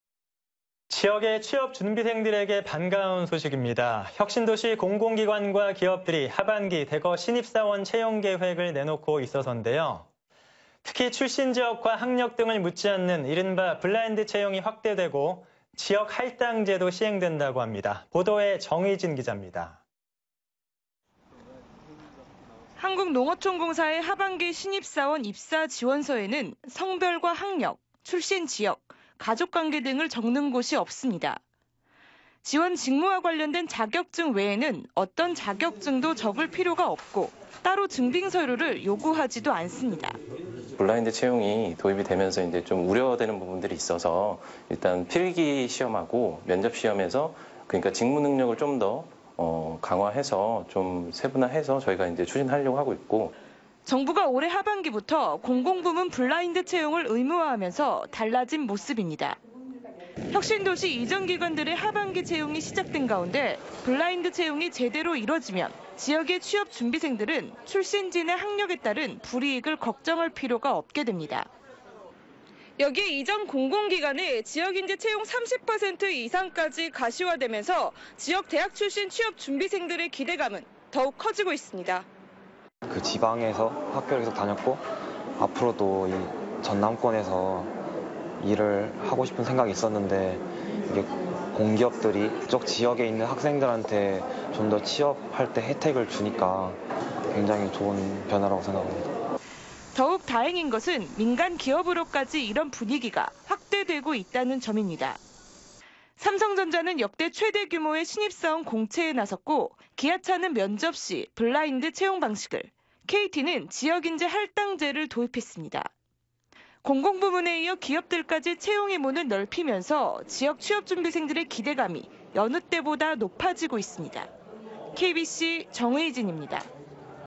혁신도시·대기업 채용 시작…지역 취준생 기대
지역의 취업 준비생들에게 반가운 소식입니다.

혁신도시 공공기관과 기업들이
하반기에 대거 신입 사원 채용 계획을
내놓고 있어섭니다.

특히 출신 지역과 학력 등을 묻지 않는
이른바 블라인드 채용이 확대고
지역 할당제도 시행된다고 합니다.

보도에 정의진 기잡니다.